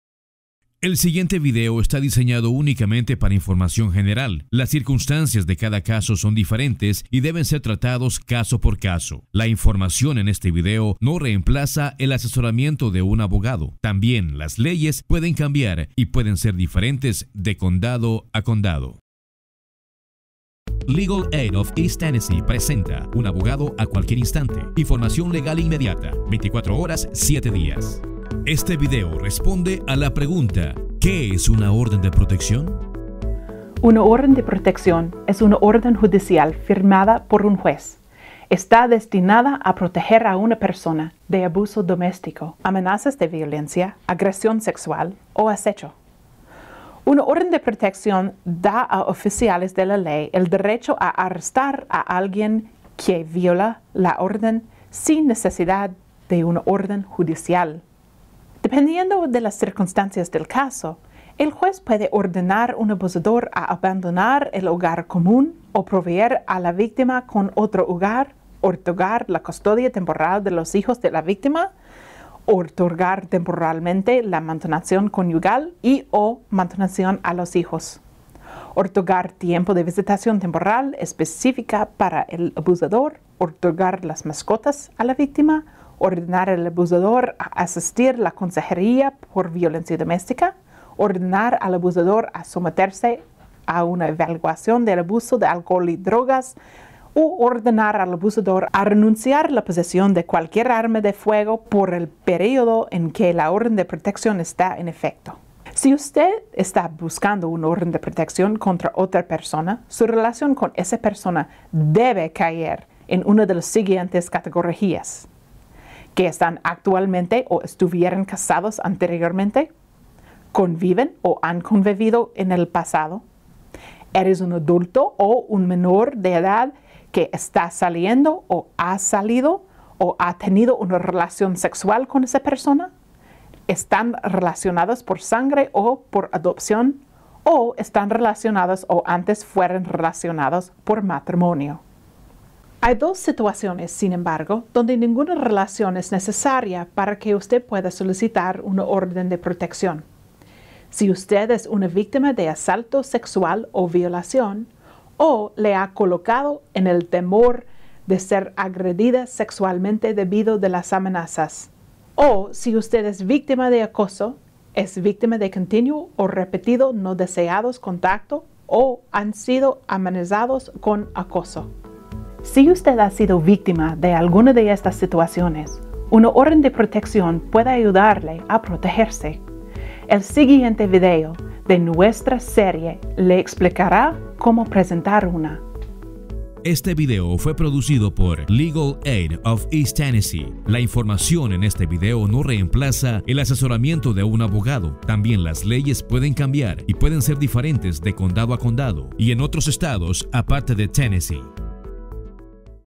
ÓRDENES DE PROTECCIÓN – Parte 1
Este video responde a la pregunta, "¿Qué es una Orden de Protección?